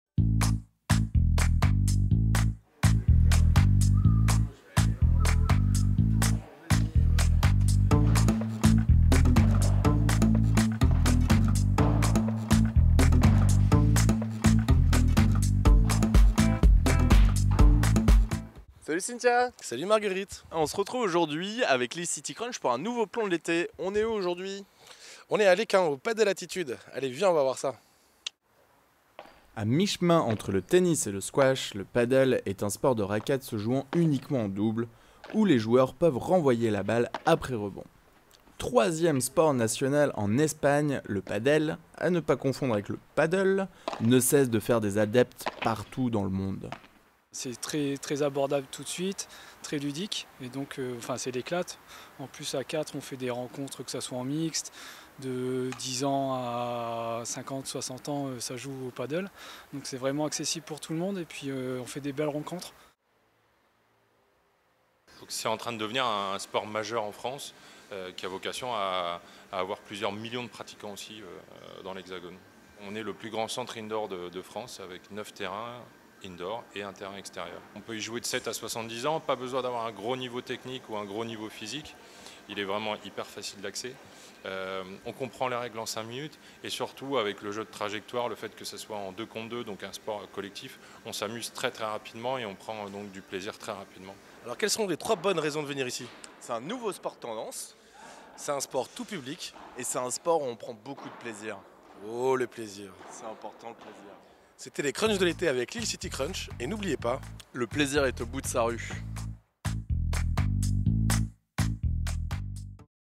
Lille City Crunch : Padel Attitude
Gauthier et Laurent nous font découvrir Padel Attitude, le plus grand centre indoor de France consacré au padel, un sport hybride entre le squash et le tennis, situé à Lesquin.